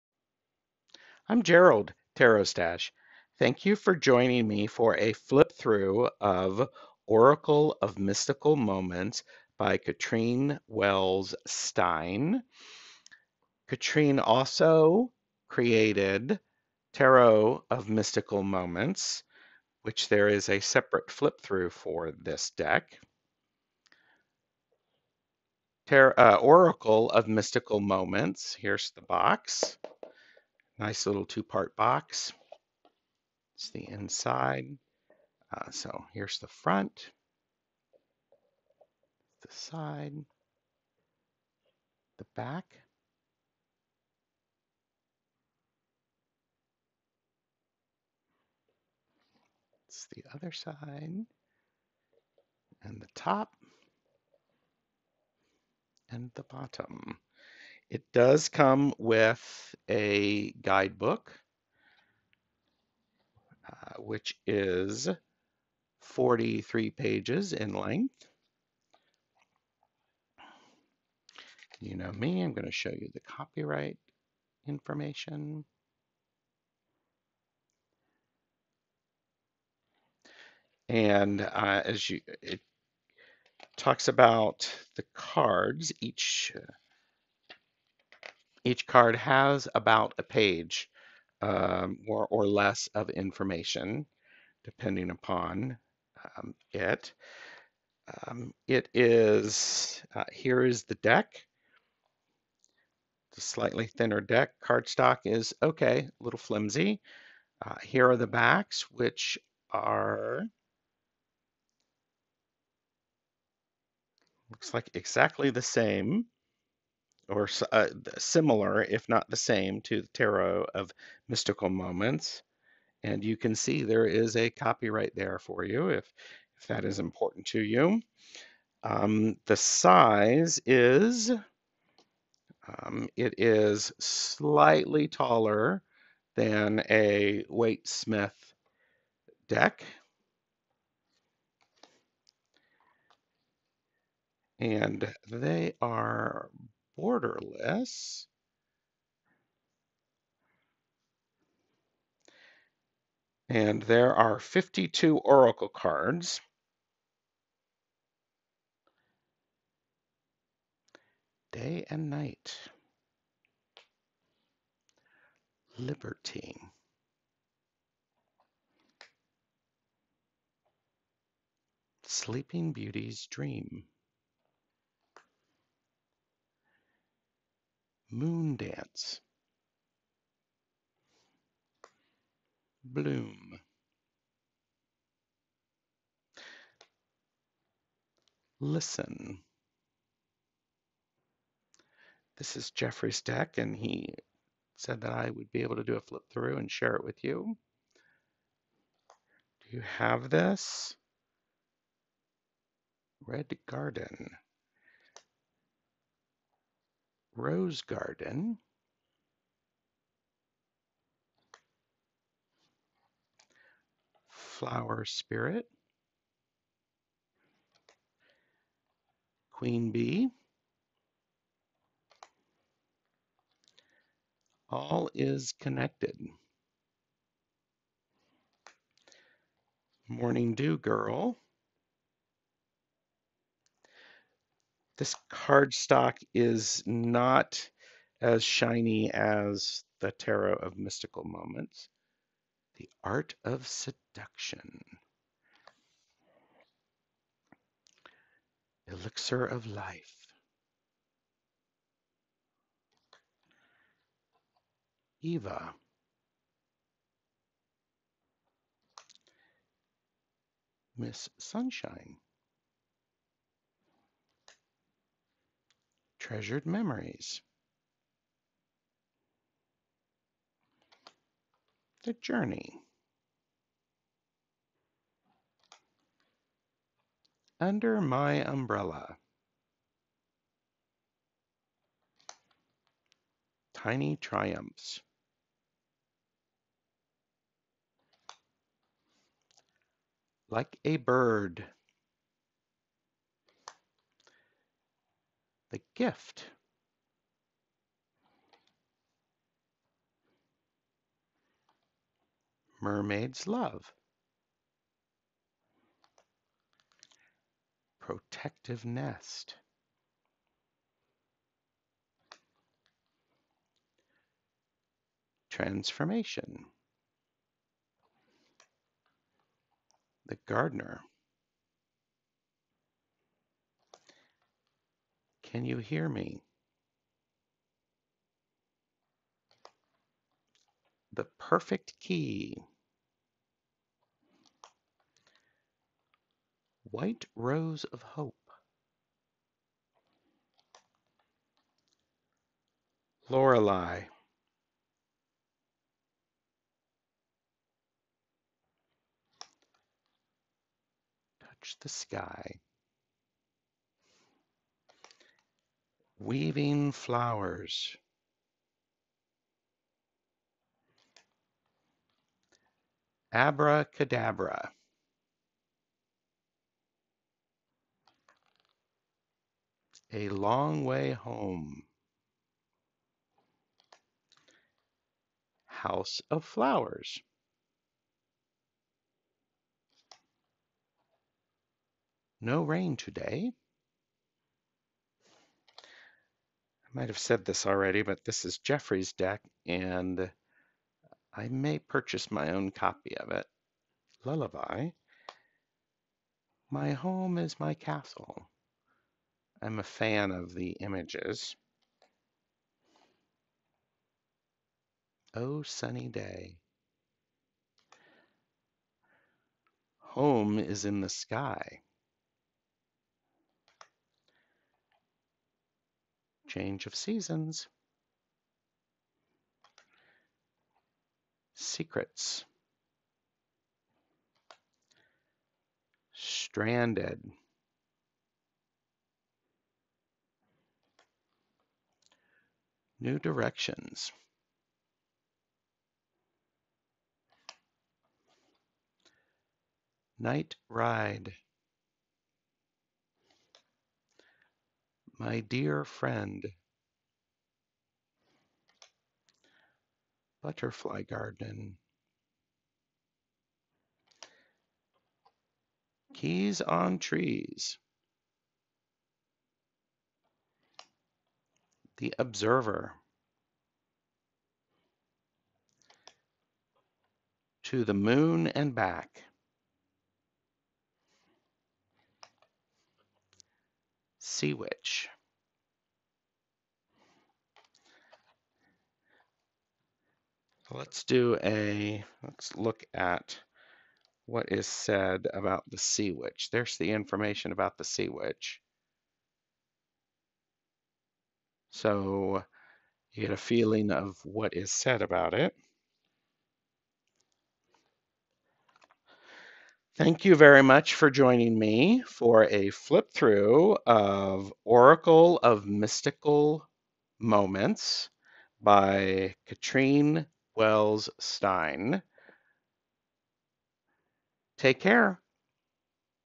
Oracle of Mystical Moments by Catrin Welz-Stein Flip Through
Flip through of Oracle of Mystical Moments by Catrin Welz-Stein published by US Games Inc. in 2017.

Catrin also created Tarot of Mystical Moments in 2020 and you can watch the flip through here. Tarot of the Mystical Moments by Catrin Welz-Stein Flip Through

My email is TarotStache at google mail.